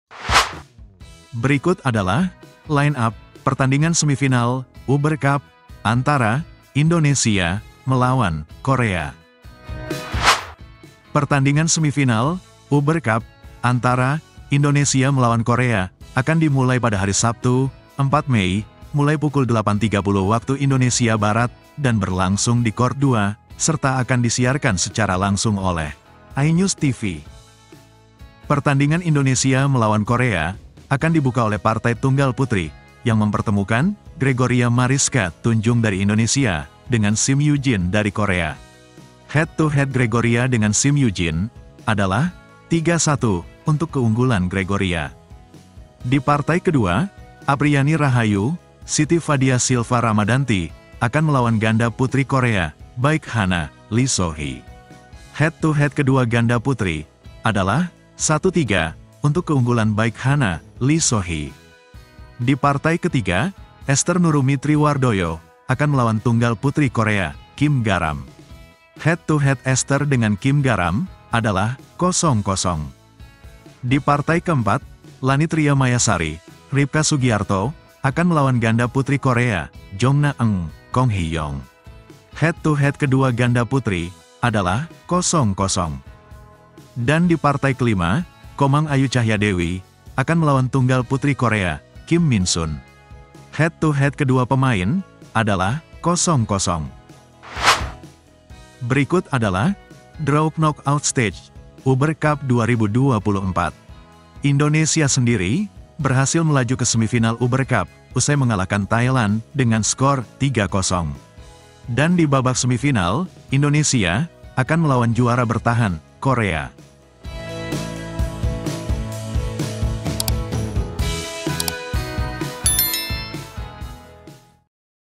Line Up Indonesia Vs Korea Semifinal Uber Cup 2024. Korea Tanpa An Se Young
Line Up Indonesia Vs Korea Semifinal Uber Cup 2024. Tim Uber Korea Tanpa An Se Young Besok Mulai Pukul 08:30 WIB Live Inews TV & Youtube BWF TV